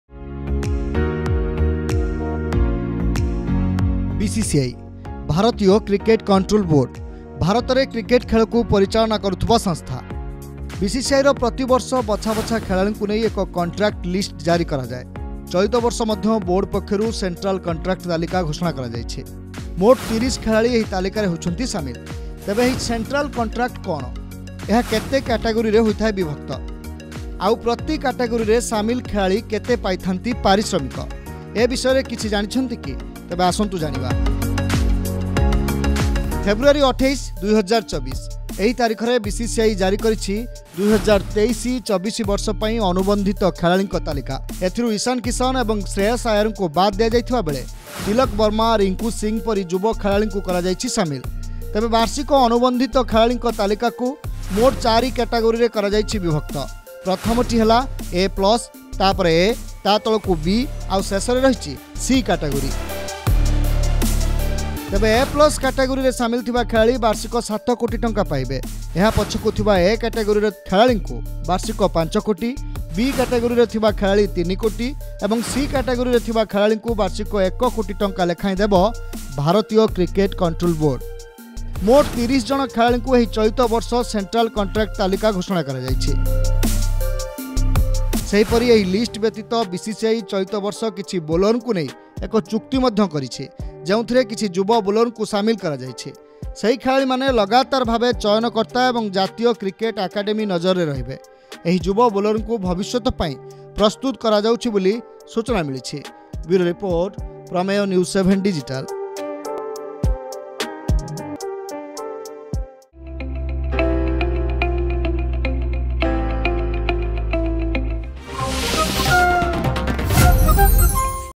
BCCI Central Contracts; Category & Salary| BCCI ପକ୍ଷରୁ କଣ୍ଟ୍ରାକ୍ଟ ତାଲିକା ଘୋଷଣା, କିଏ ପାଇବେ କେତେ ଦରମା?
BCCI Central Contracts; Category and Salary as per Grade | BCCI ପକ୍ଷରୁ ସେଣ୍ଟ୍ରାଲ କଣ୍ଟ୍ରାକ୍ଟ ତାଲିକା ଘୋଷଣା, ଜାଣନ୍ତୁ କେଉଁ ଗ୍ରେଡ଼ ଖେଳାଳି ପାଇବେ କେତେ ଦରମା...